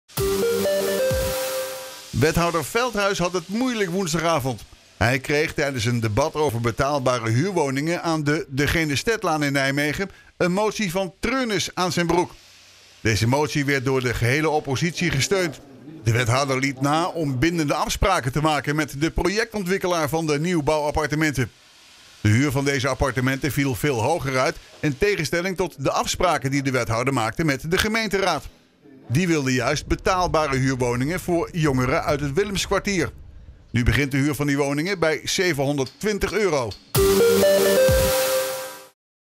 Wethouder Velthuis onder vuur om te dure starterswoningen
NIJMEGEN - Oppositiepartijen hebben woensdagavond onder leiding van Giselle Schellekens (PvdA) in de raadsvergadering een motie van treurnis ingediend over het handelen van wethouder Bert Velthuis (SP) inzake starterswoningen aan de Genestetlaan. De wens van de raad en de wethouder was om daar betaalbare woningen te bouwen. Het resultaat is echter bijzonder kleine woningen waarvoor projectontwikkelaar Stuworld voor de goedkoopste 710 euro huur exclusief servicekosten vraagt. De oppositie verwijt Velthuis dat hij zijn toezeggingen om zich hard te maken voor betaalbare starterswoningen niet is nagekomen. Enkel de coalitiepartijen steunden de motie niet, waarmee deze met een kleine meerderheid verworpen is.